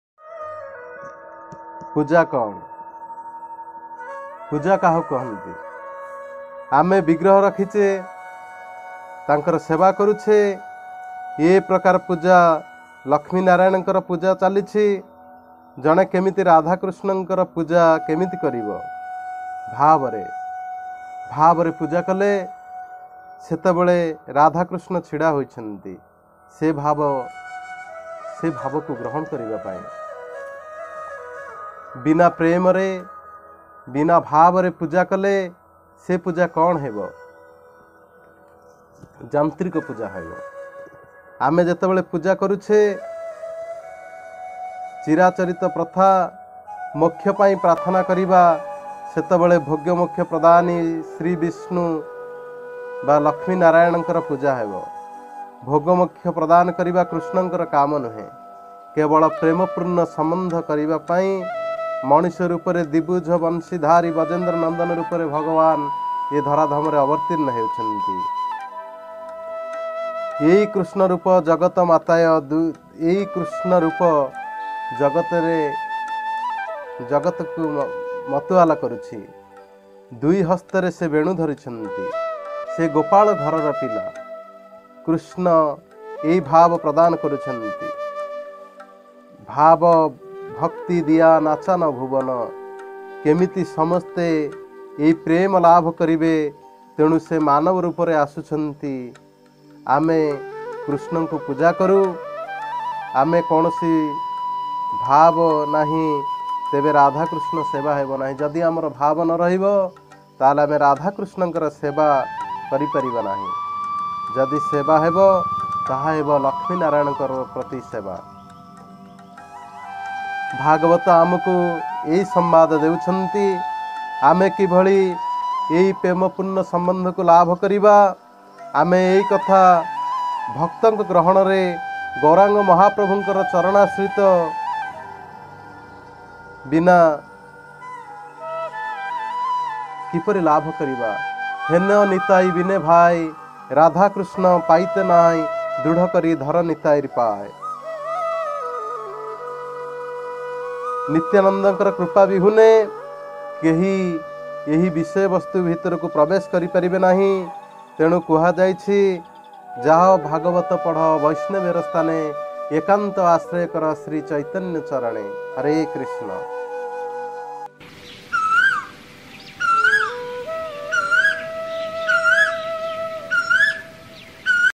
କୃଷ୍ଣ ସମ୍ବନ୍ଧ ଭାଗ -୦୫
Relationship with Krishna part -05
what is worship???
all story we Listen from lotus mouth of Sri Guru Maharaj
Hare Krishna
your servant Mukundadatta Das